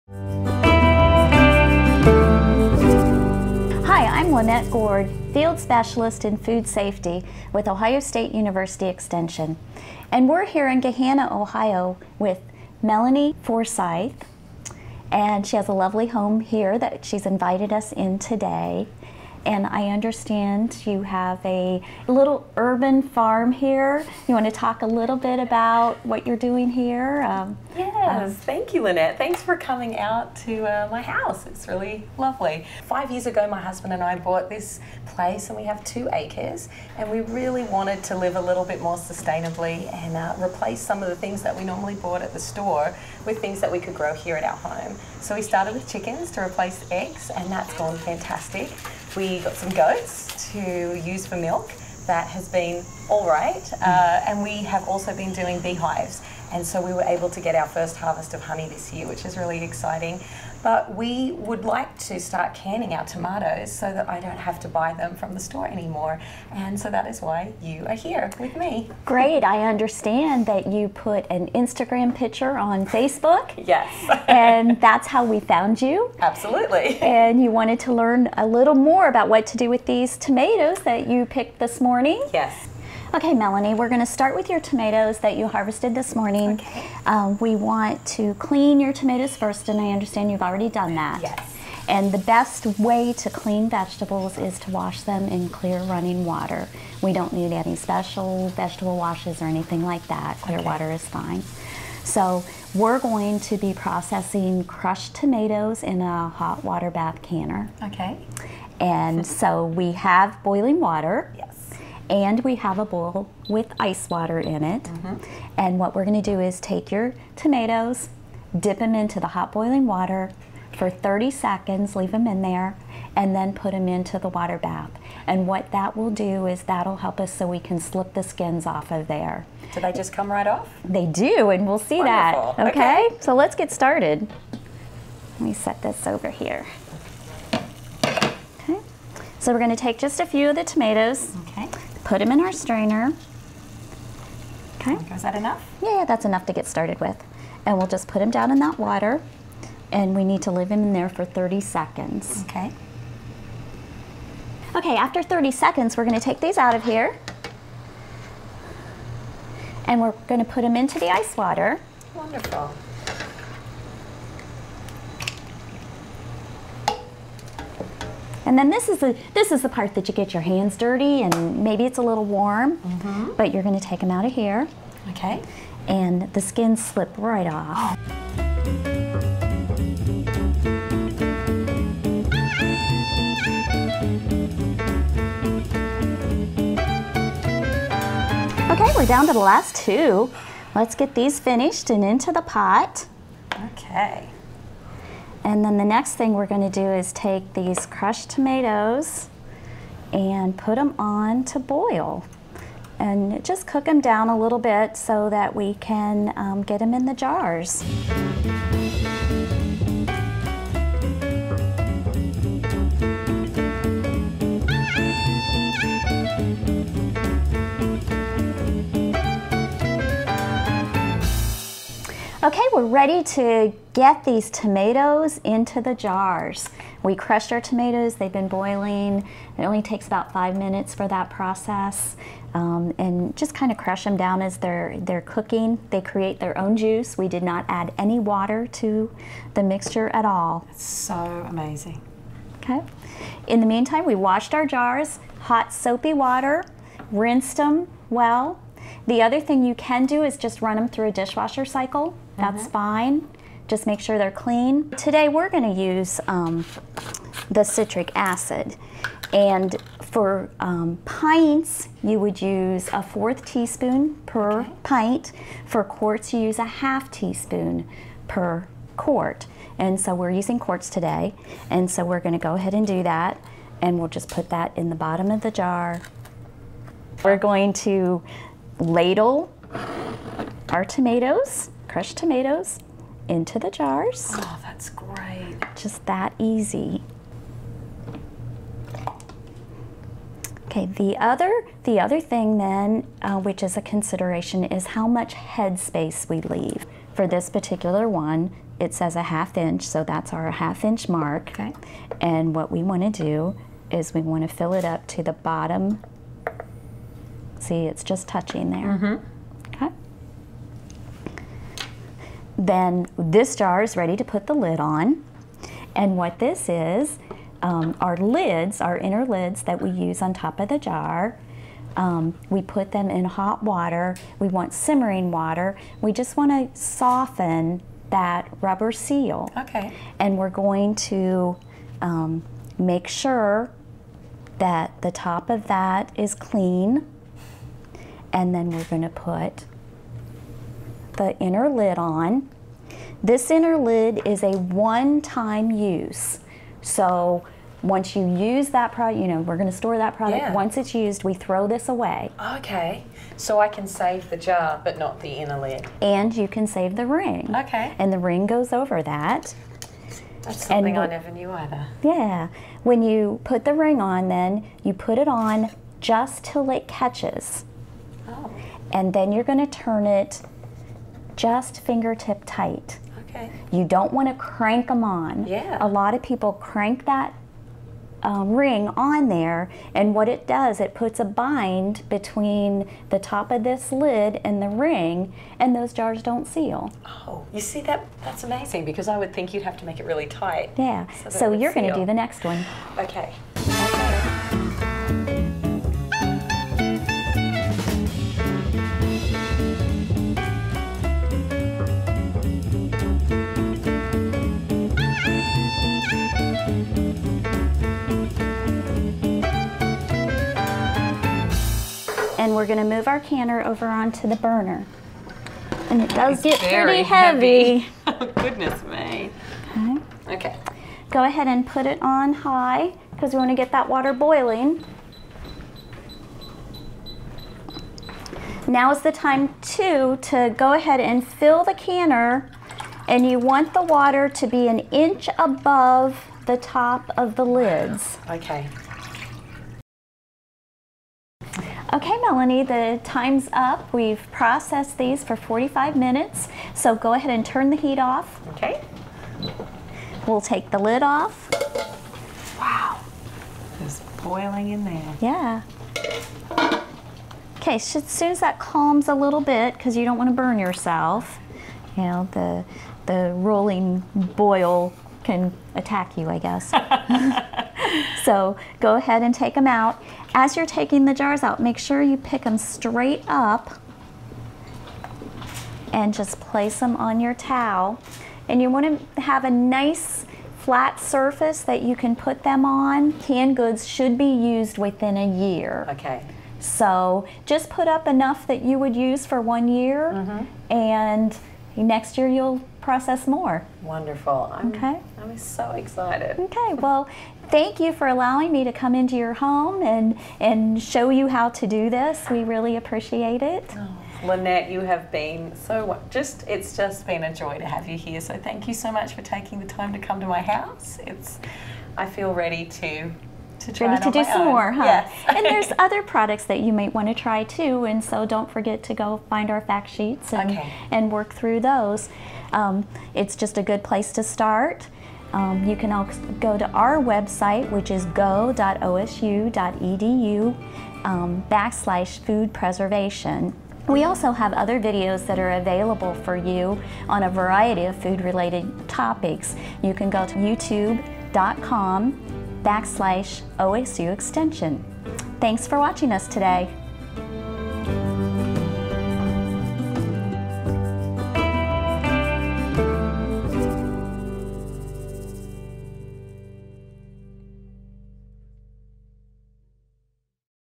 How to Can Tomatoes in a Water Bath Canner
Growing and canning your own tomatoes can be a fun and economical way to have nutritious locally grown food year-round.  Ohio State University Extension State Food Safety Leader Linnette Goard teaches a novice canner how to safely can tomatoes.
For this and other canning recipes go to:
go.osu.edu/foodpreservation